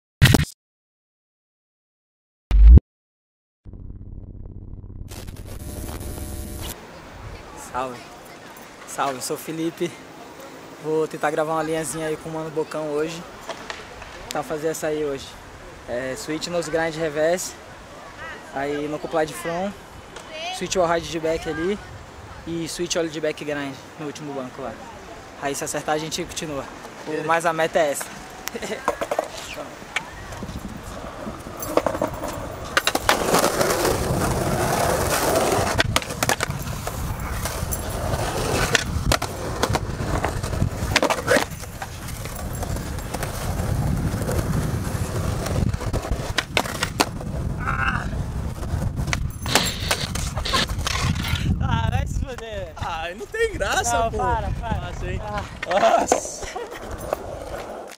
Timeline #02: Felipe Oliveira
TIMELINE, a nova webserie da SAT inspirada no impulso sequencial da força e na execução de manobras consecutivamente. Algo perto de uma verdadeira coreografia montada estrategicamente para conquistar esse precioso resultado, ate deixar uma linha no tempo...
Desta vez Felipe Oliveira conseguiu voltar de prima! algo atípico dentro do mundo das estatísticas.
Filmado e editado por Rodrigo Andrade (Bocão)